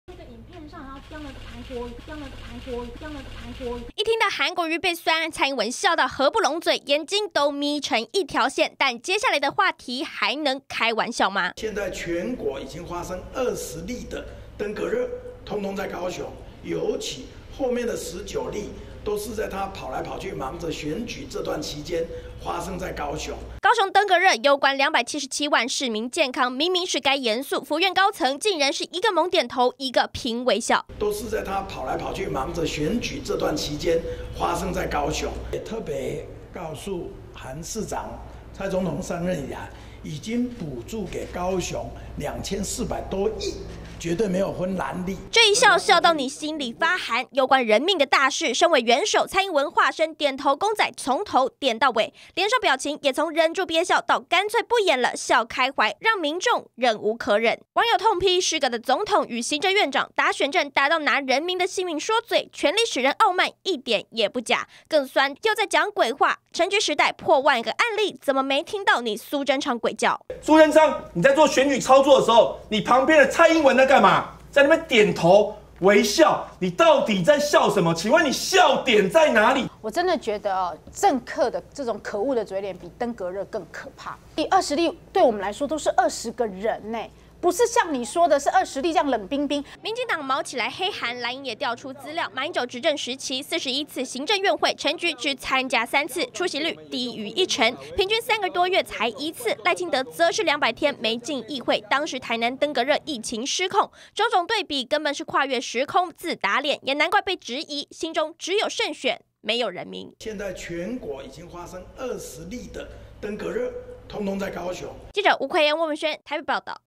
20190616中天新聞 扯！ 談港都登革熱20例 蔡蘇狂點頭竟還「微笑」
高雄市長韓國瑜日前北上參加行政院會，卻被行政院長蘇貞昌，在粉專po出剪輯影片抹黑，被媒體詢問時，一旁的蔡總統聽到「韓國瑜被酸」關鍵字，竟然「笑開懷」，更離譜的是，提到高雄登革熱20案例，討論的明明是嚴肅話題，還攸關人命，蔡蘇兩人卻是狂點頭還露出微笑，藍營痛批，府院心中只有「黑韓」沒有人民，民眾也狠酸，跟本是「失格的總統和行政院長」，打選戰打到拿人民的性命說嘴太過分。